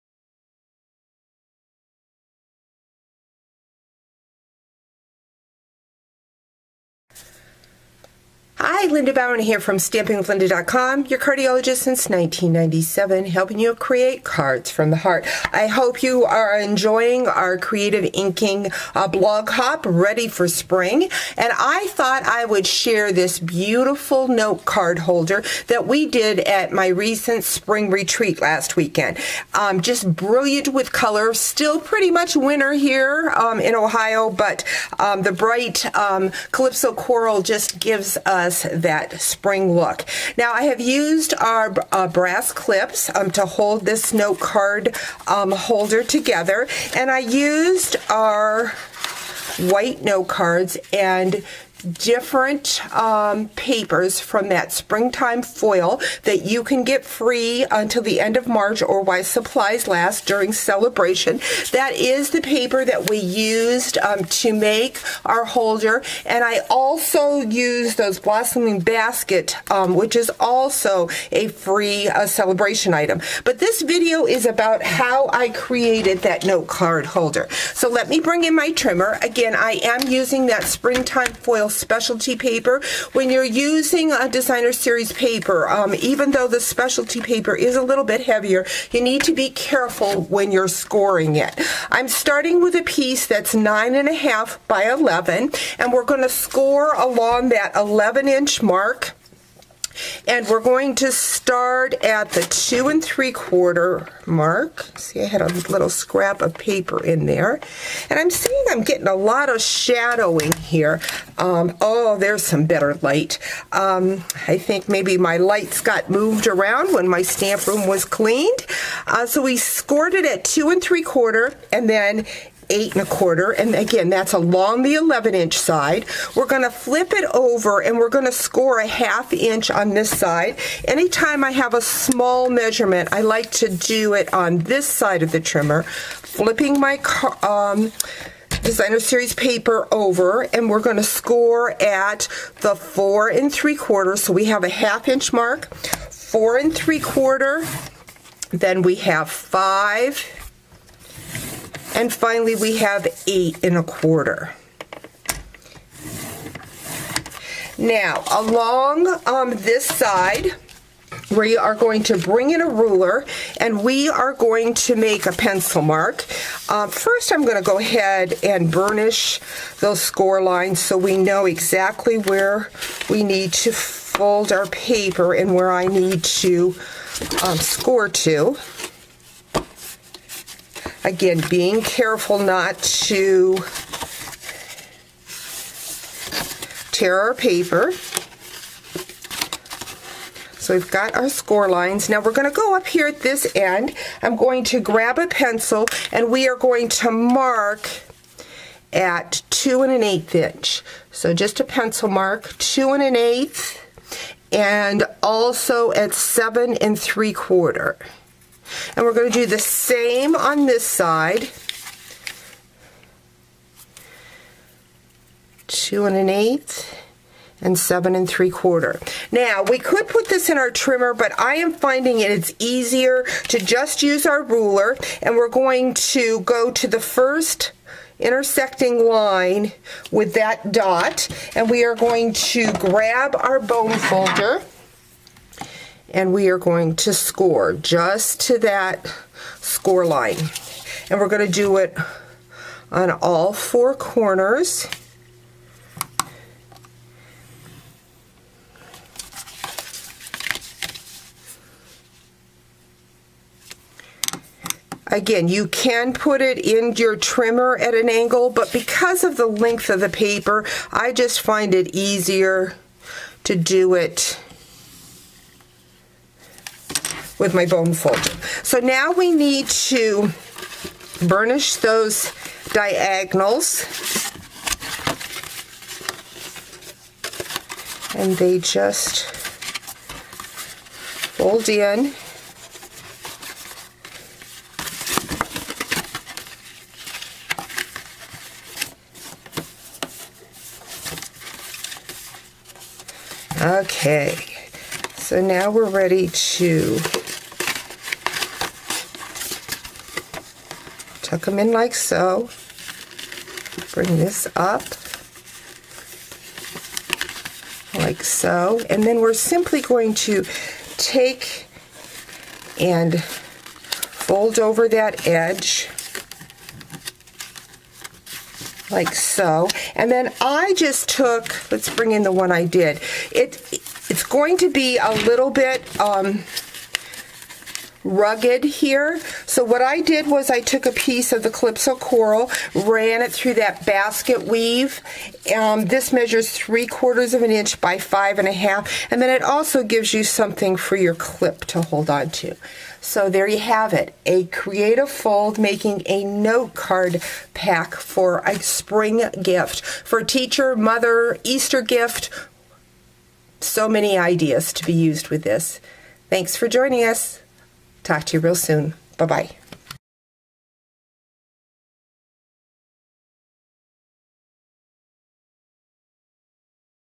Springtime Note Card Holder
The Springtime Specialty Designer Series and the Blossoming Basket Bundle from Stampin' Up! made this a very elegant Note Card Folder.